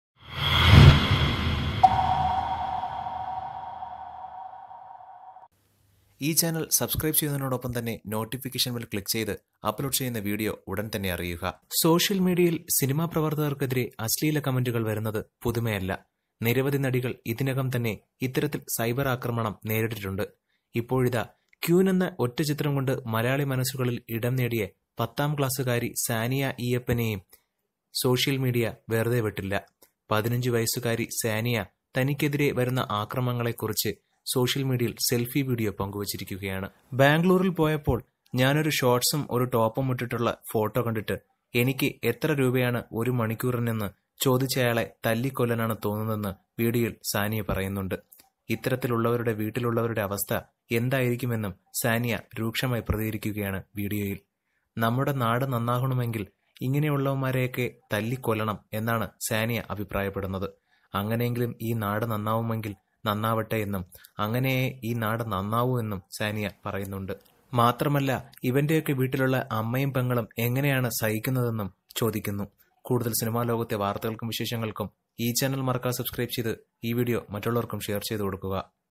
ഒരു മണിക്കൂറിന് നടിയുടെ വില ചോദിച്ച വ്യക്തിക്കെതിരെ നടി രംഗത്ത് || actress saniya || saniya eyappan
ഒരു മണിക്കൂറിനു ക്വീൻ നായികയുടെ വില ചോദിച്ച വ്യക്തിക്കെതിരെ നടിയുടെ പ്രതിഷേധം
ഇത് കേട്ട് നോക്കൂ